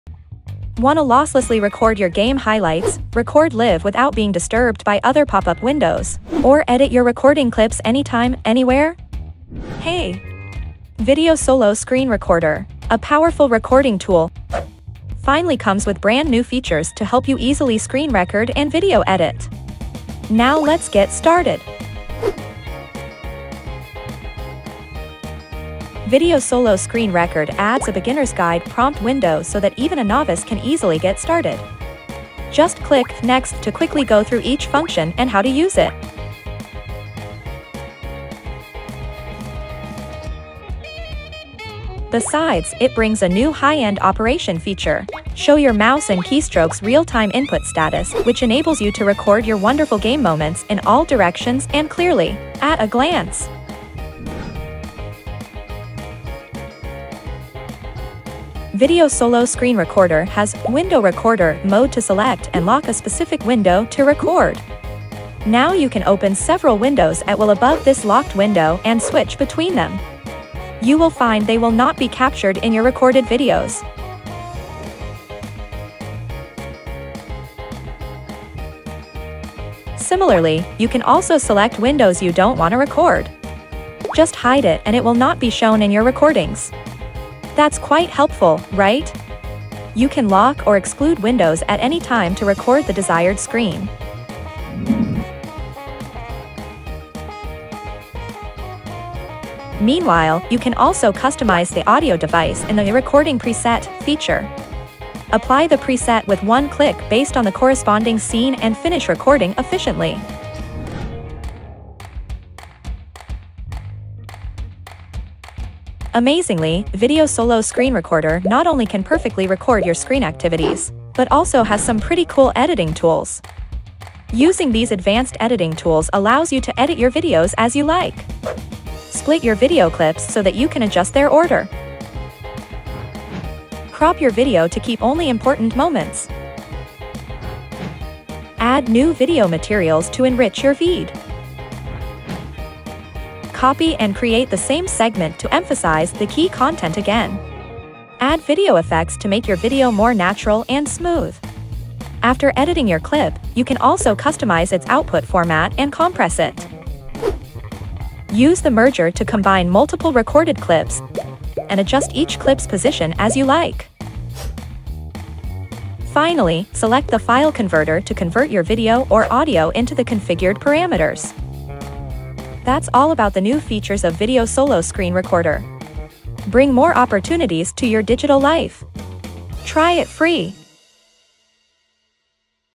[2023] What's New in VideoSolo Screen Recorder 1.2.60 Upgrade : Unlock All the New Features!
Hi, guys! In today's video, we'll show you What's new in this latest VideoSolo Screen Recorder V1.2.60. Have a quick look😆

There are 6 New Features in VideoSolo Screen Recorder 1.2.60 Windows Version:
✅ Beginner's Guide Window
✅ Show Mouse & Keystroke Real-time Input Status
✅ Lock a Specific Window to Record
✅ Exclude Specific Windows
✅ Customize the Audio Device
✅ Editing Tools  (Trim, compress, merge and convert video/audio）

⌚ Timestamps
0:00 Introduction
0:12 VideoSolo Screen Recorder
0:18 New Features V1.2.60
0:25 Beginner's Guide Window
0:38 Show Mouse & Keystroke Real-time Input Status
0:57 Lock a Specific Window to Record
1:17 Exclude Specific Windows
1:38 Customize the Audio Device
1:53 Editing Tools
2:02 Trim, compress, merge and convert video/audio
2:57 Outro